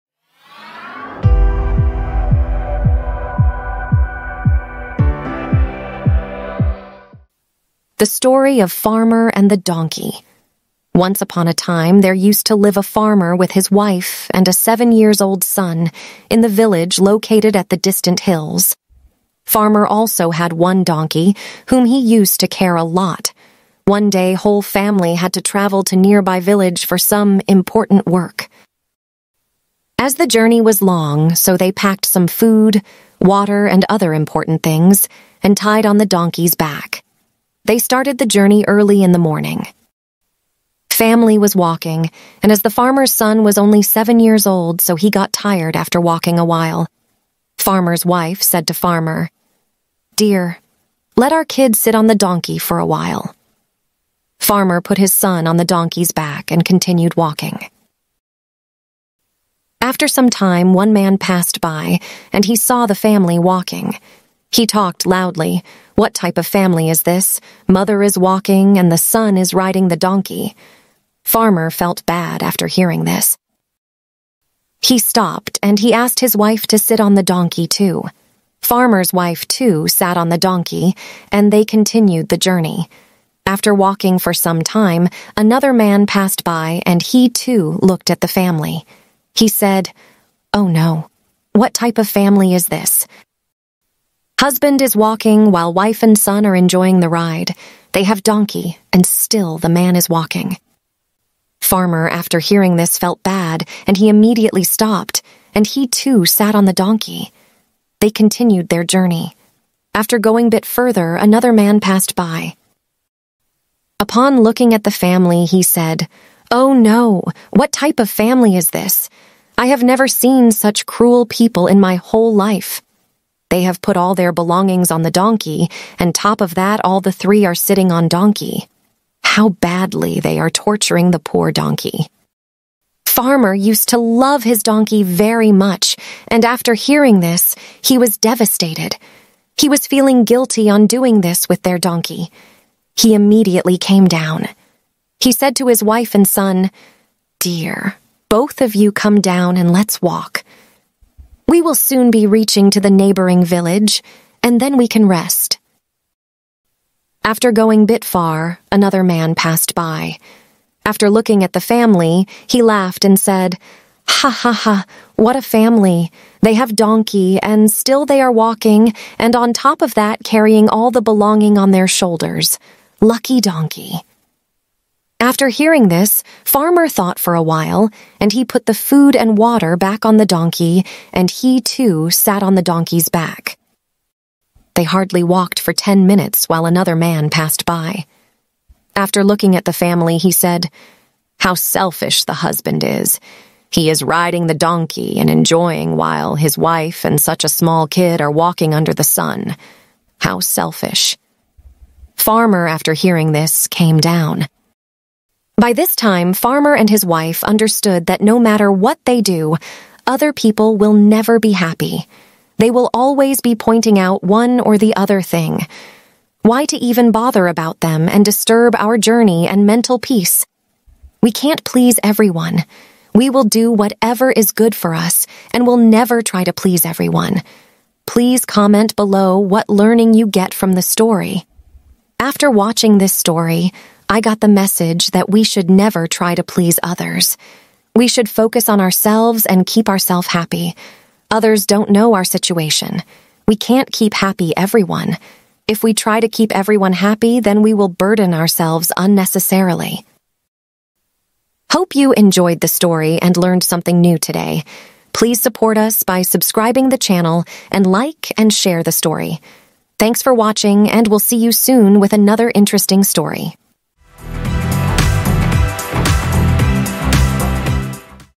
The Farmer and His Donkey | Animated Story for Kids | Wisdom
This story is about the Farmer and his Donkey. They set out on a journey and received comments from other people. To know what happened watch the story.
animated story for kids.